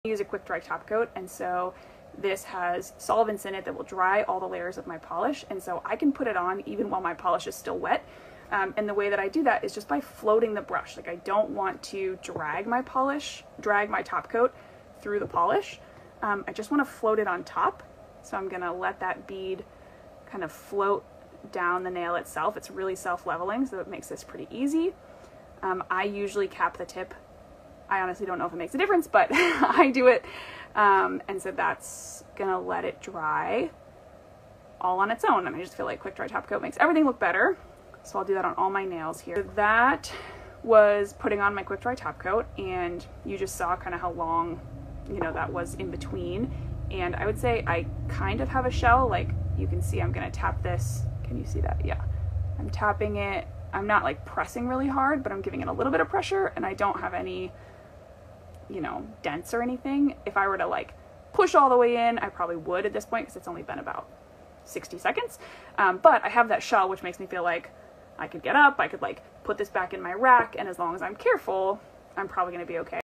How to apply Quick Dry Top Coat
Products used: Quick Dry Top Coat and Zinc (sage green polish) both by Olive Ave Polish

Follow Olive Ave Polish on instagram @oliveave_polish to see more colors, comparisons, and behind the scenes!